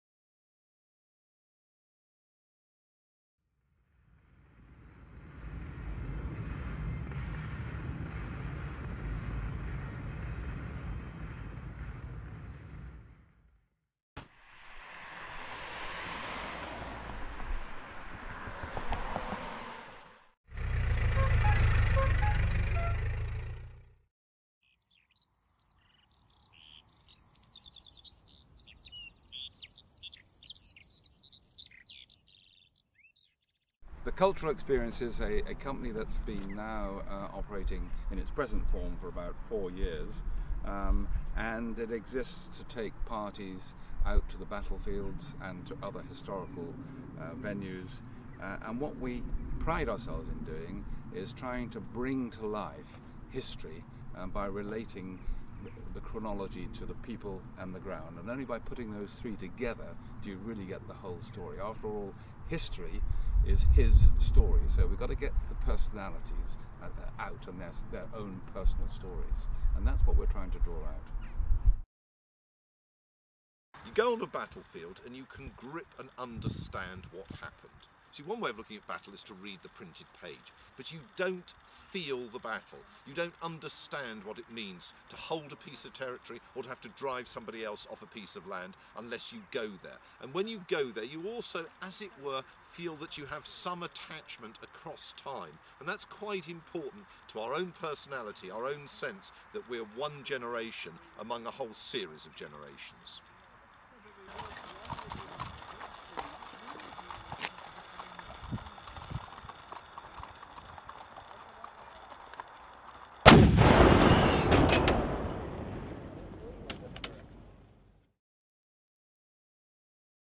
The Cultural Experience on the Battlefield of Waterloo
Major-General Ashley Truluck & Professor Jeremy Black explain the ethos behind The Cultural Experience and the importance of visiting iconic battlefields and historic locations.